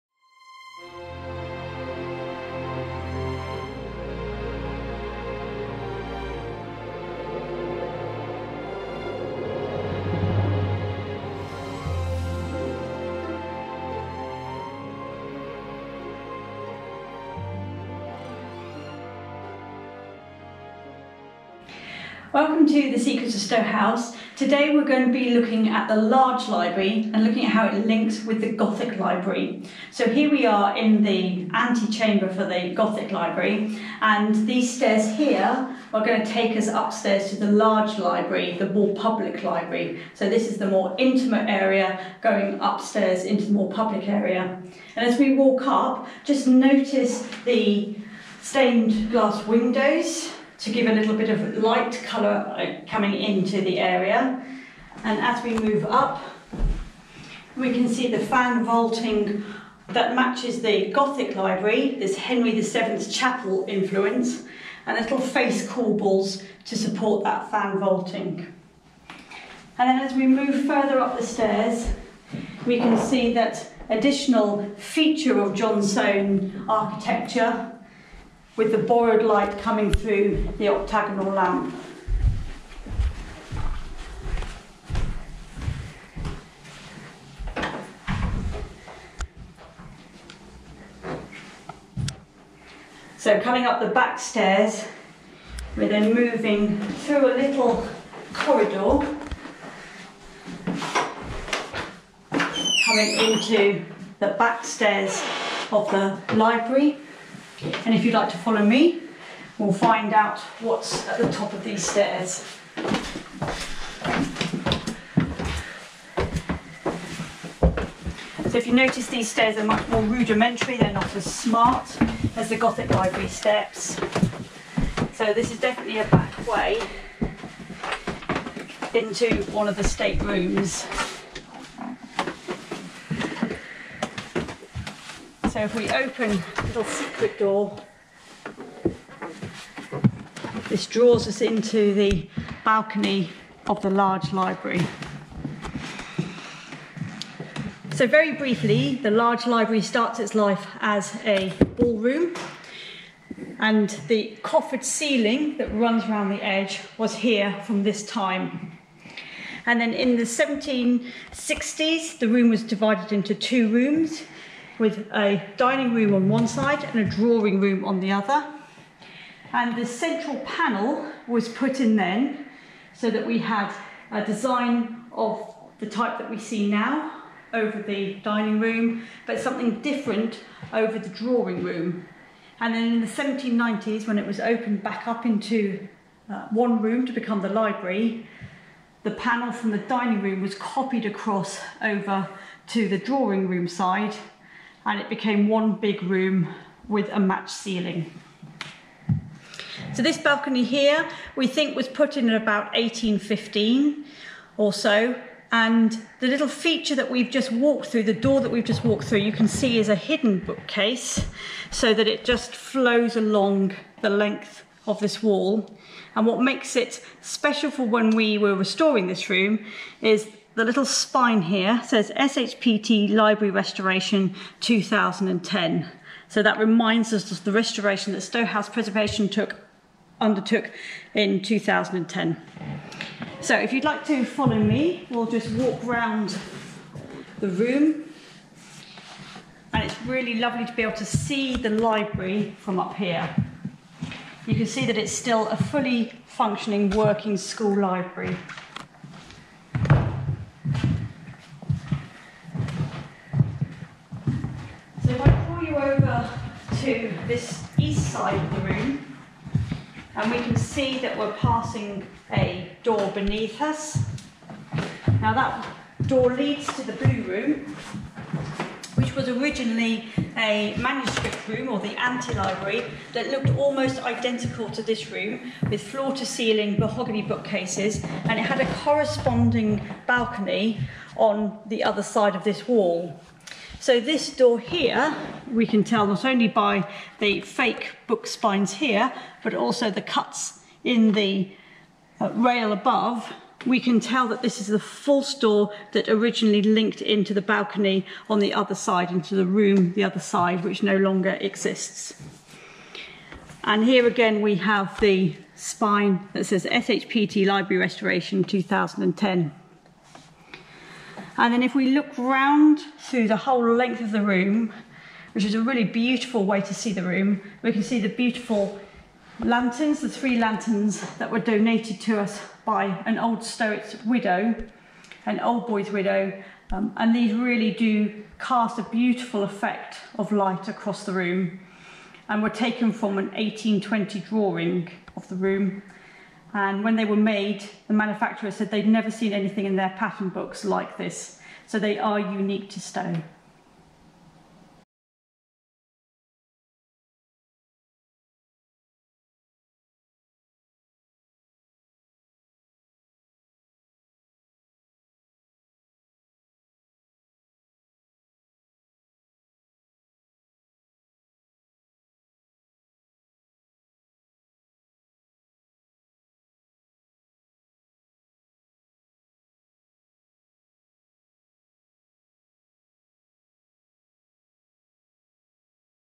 04 Secrets of Stowe House - The Large Library
Join Anna McEvoy our House Custodian today as she takes around the large Library in this episode of our Secret of Stowe series. From secret doors to bespoke lamp fittings, this episode is takes us behind-the-scenes and into hidden spaces previously accessible to visitors! Discover some of the fascinating architectural quirks that tell the unique story of Stowe House.

for more information about Stowe House.